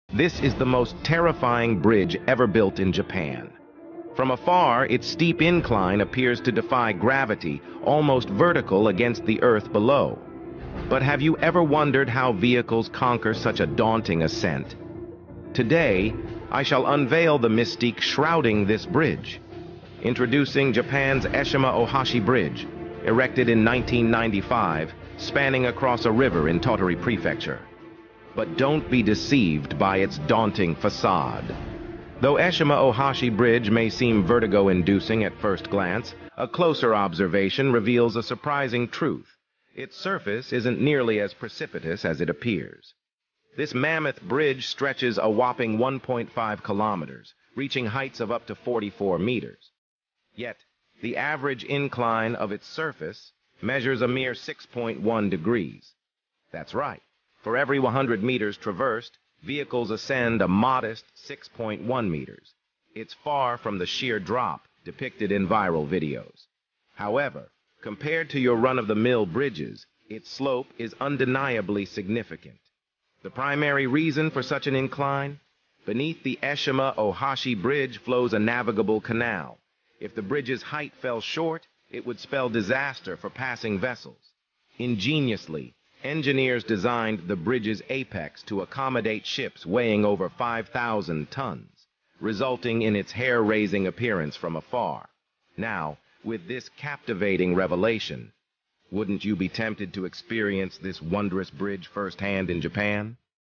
The most terrifying bridge in Japan - The Eshima Ohashi Bridge
The most terrifying bridge in Japan- The Eshima Ohashi Bridge. Would you dare to drive through?

Loved this video? Let's grow the Quick Bites community together!